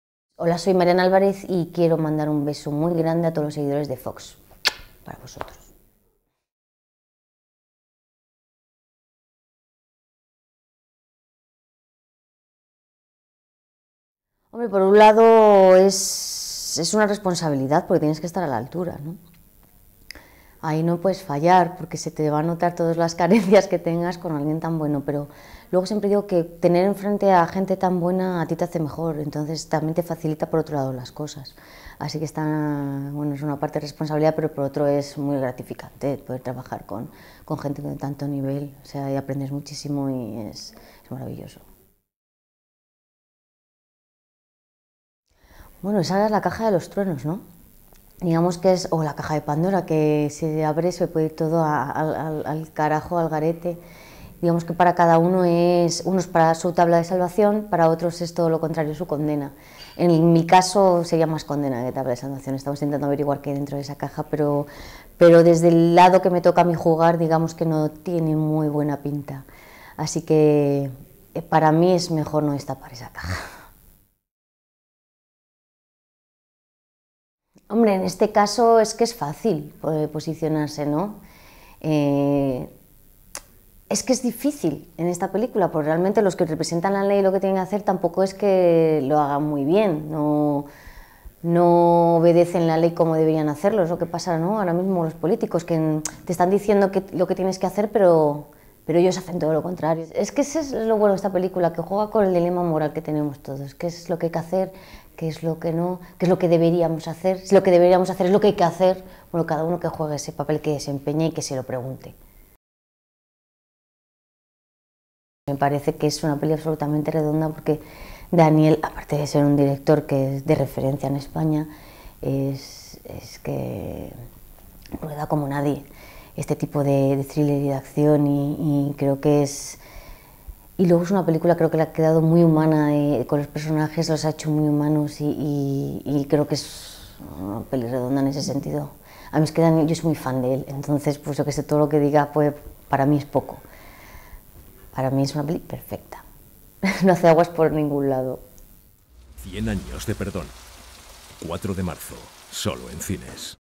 CIEN AÑOS DE PERDÓN | Entrevista Marian Álvarez | Ya en cines
Una mañana lluviosa. Seis hombres disfrazados y armados asaltan la sede central de un banco en Valencia. Lo que parecía un robo limpio y fácil pronto se complica y nada saldrá como estaba planeado. Esto provoca desconfianza y el enfrentamiento entre los dos líderes de la banda, El Uruguayo y El Gallego. Pero ¿qué es exactamente lo que buscan los atracadores? CIEN AÑOS DE PERDÓN es el nuevo thriller de Daniel Calparsoro ¡4 de marzo en cines!